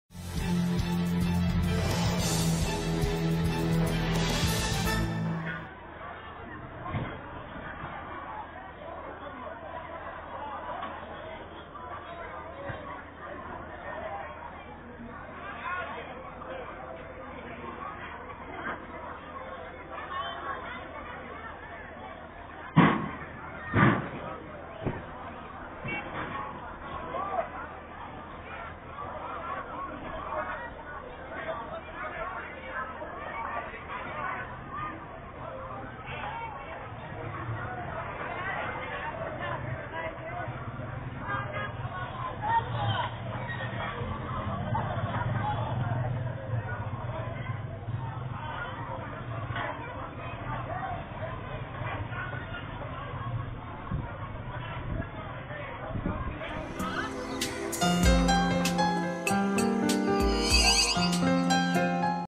رصد | مشاجرات بين الاهالي بقرية صفط جدام بتلا بسبب نقص انابيب البوتجاز
تشهد قرية صفط جدام التابعة لمركز تلا بالمنوفية ازمة حادة فى نقص اسطوانات الغاز مما دفع الاهالى الى التزاحم على مستودعات التوزيع زنشبت مشاجرات بينهم فيما نفى محافظ المنوفية الانقلابى وجود اى ازمة